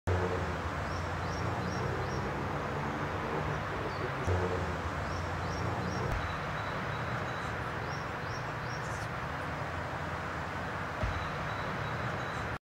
2 Fuchswelpen auf dem Friedhof - fox puppies in cemetery
...  gut getarnt und von den Friedhofsbesuchern selten entdeckt.